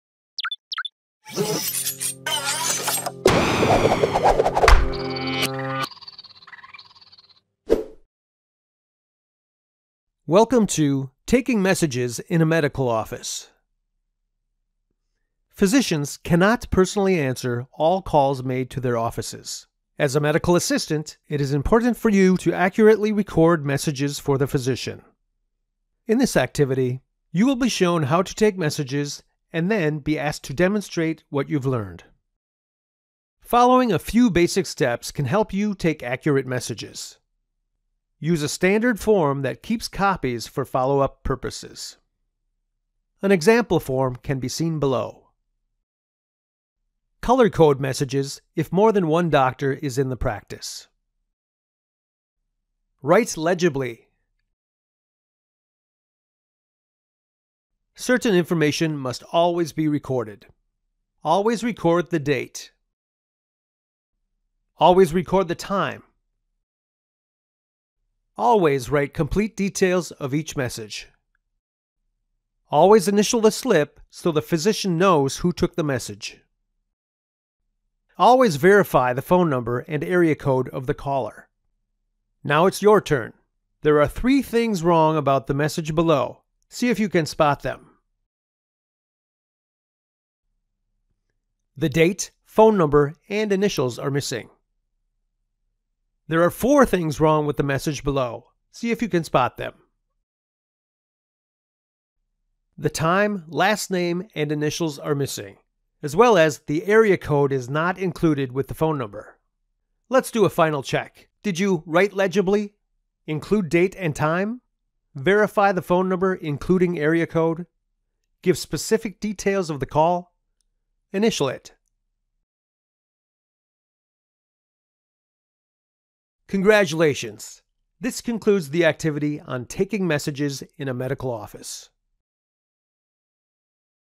Taking Messages in a Medical Office
In this screencast, people preparing to work in a medical office review the basic steps to follow when taking phone messages.

Credits:
    - Content Author: Barb Portzen